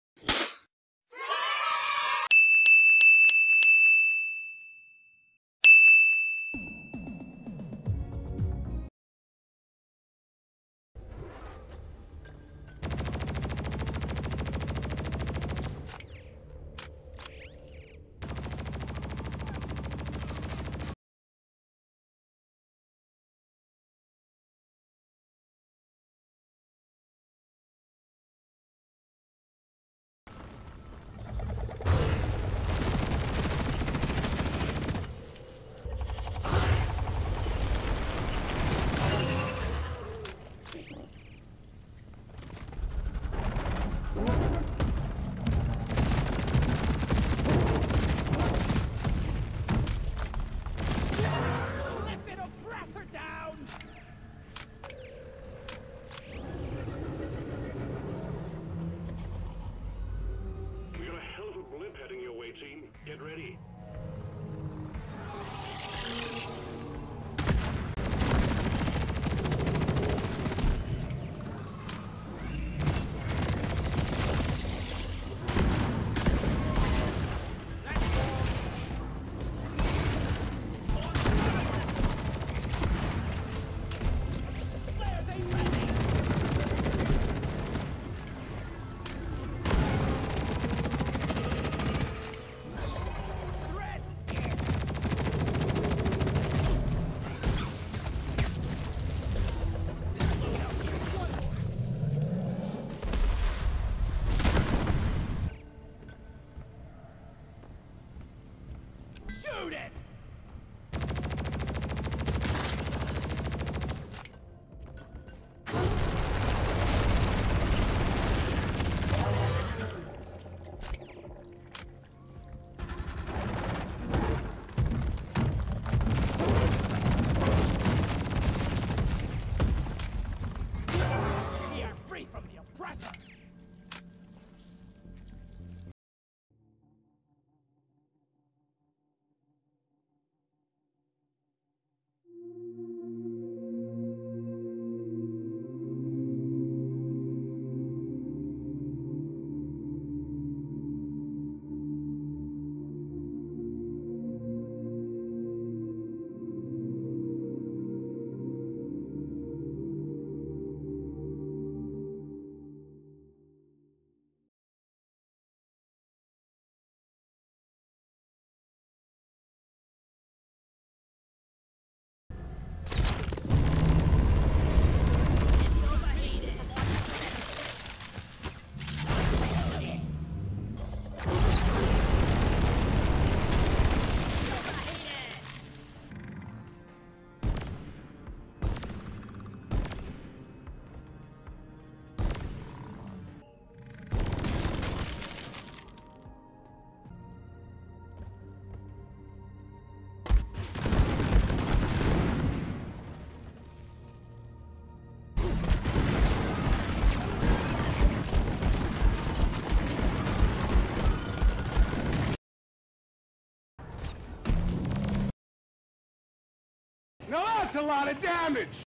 Giving Hyperalloy Assembly another try and compare with EM Refire Booster
Thanks you everyone for supporting me reaching 100 subscribers threshold! maybe we could reach further helping me speak in video more comfortably

Many comments want me to give Hyperalloy Assembly a re-try so i did, as comment said and some Trivia from comments at the near end of the video!

Time Stamp:
0:00 - Intro
0:30 - Hyperalloy Assembly
0:57 - That guy on the chair gave me a fun time
1:54 - EM Refire Booster
2:19 - Result
2:50 - Other Trivias
3:36 - THATS ALOT OF DAMAGE